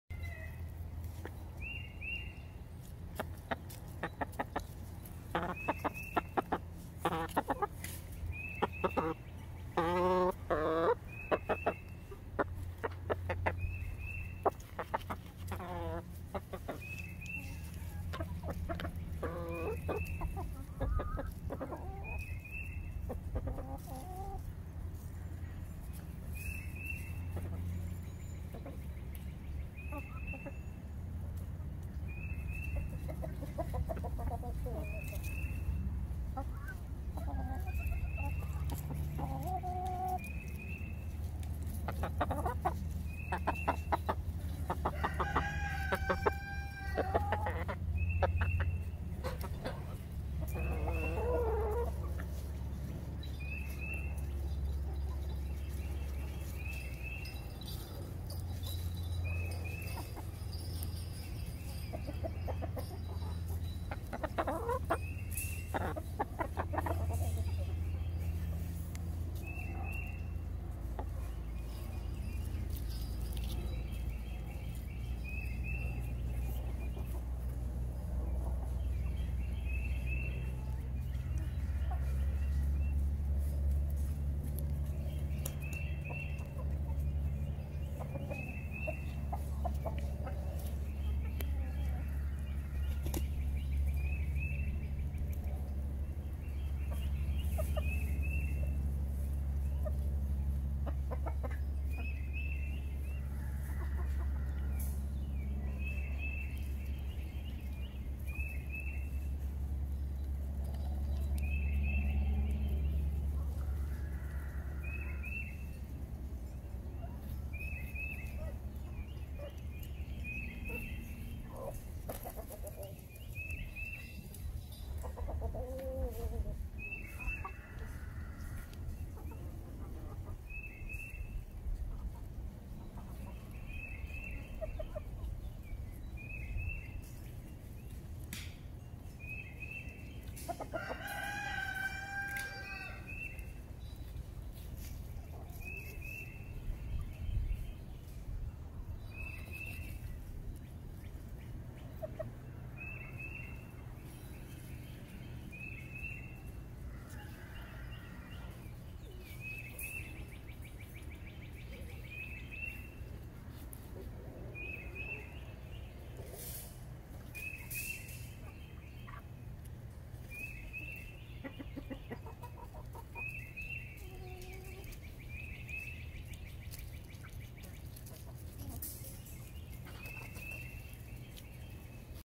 Sounds of the Farm - ASMR style - relaxation vol. 46
Relax during the pandemic quarantine. Let the sounds of the farm take you to a state of peace and tranquility.

Scenes from the Hurt Hacienda in Sherman, TX.